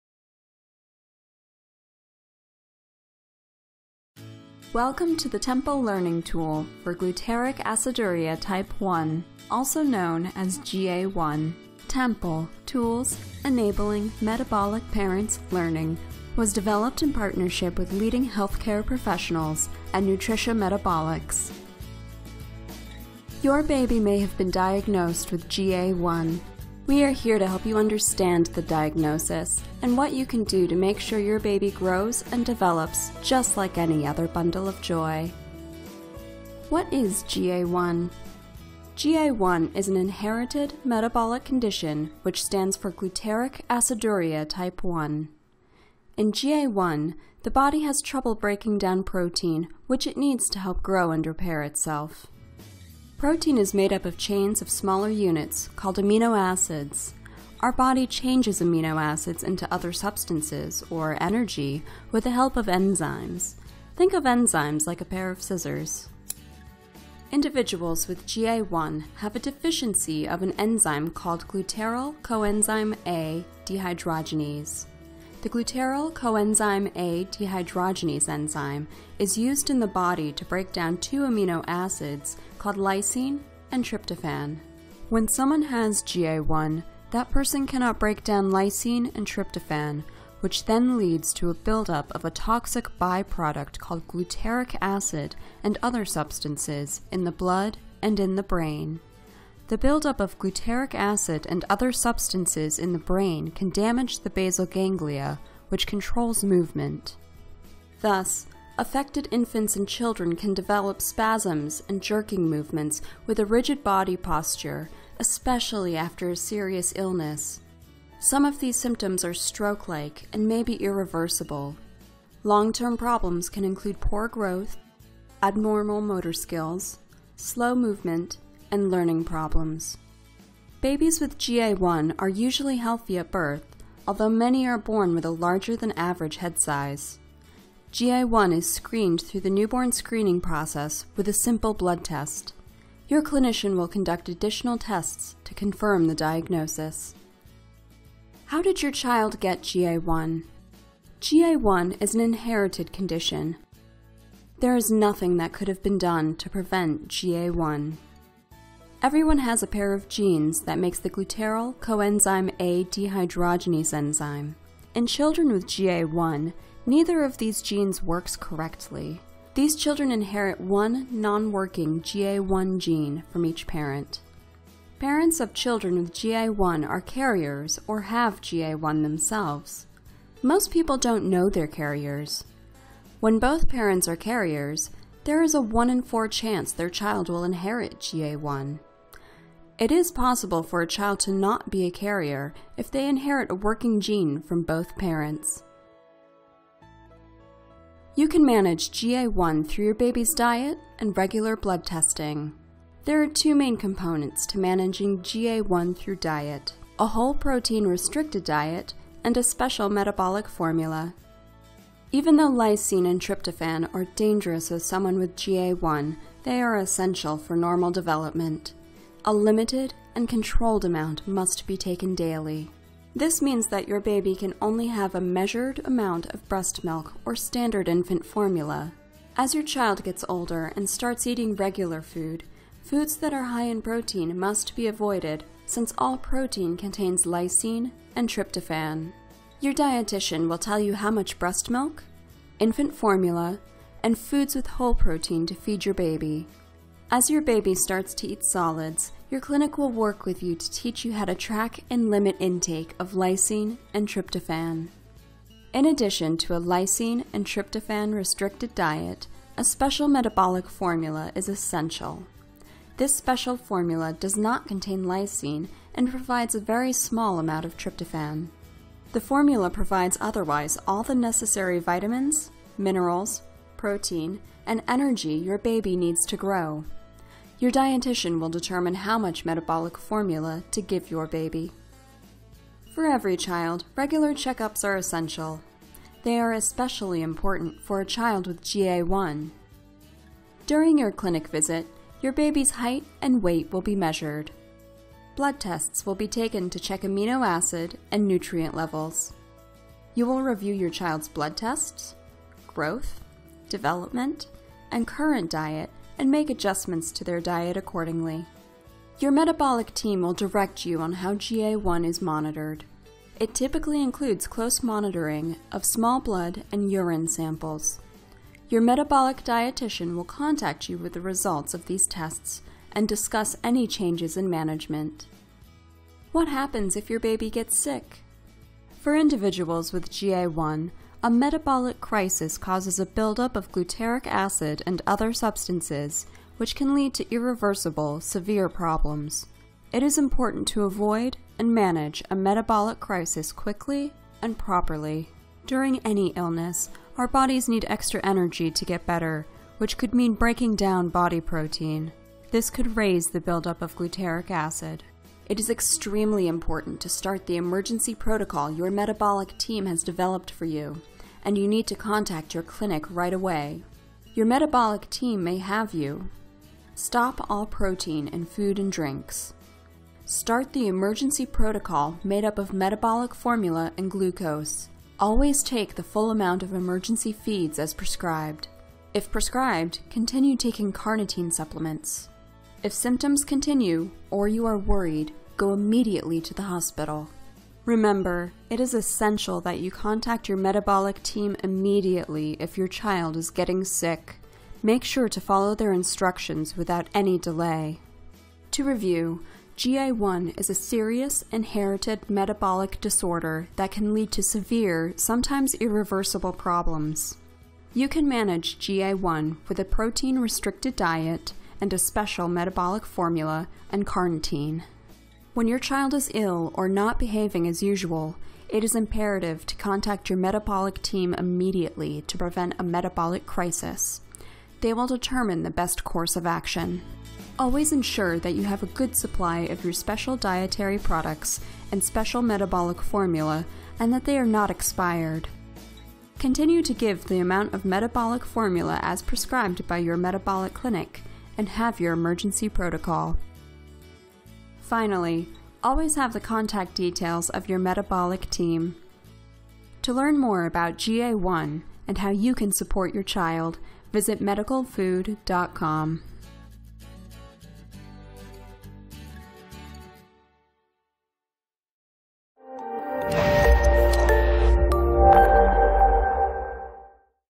Glutaric aciduria type 1 (GA-1)
Easy-to-follow educational tool for families with infants diagnosed with glutaric aciduria type 1 (GA-1). The video shares the basics of what the disorder is, how to properly manage it, and the genetics behind the disorder.
Please note that MedicalFood.com is now called NutriciaMetabolics.com.